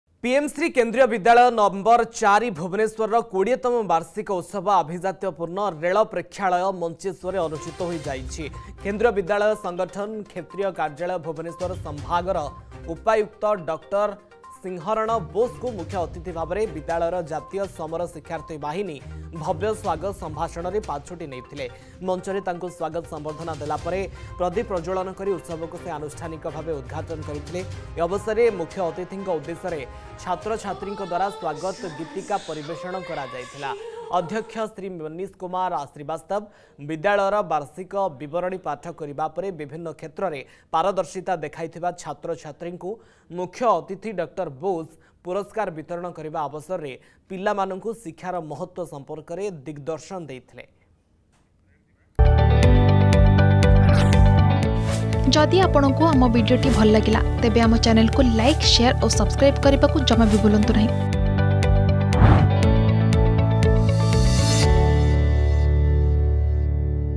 ପୀଏମ ଶ୍ରୀ କେନ୍ଦ୍ରୀୟ ବିଦ୍ୟାଳୟର 20ତମ ବାର୍ଷିକ ଉତ୍ସବ ପାଳନ || Annual Celebration
ଭୁବନେଶ୍ୱରର ମଞ୍ଚେଶ୍ୱରରେ ପୀଏମ ଶ୍ରୀ କେନ୍ଦ୍ରୀୟ ବିଦ୍ୟାଳୟର 20ତମ ବାର୍ଷିକ ଉତ୍ସବ ପାଳନ କରାଯାଇଛି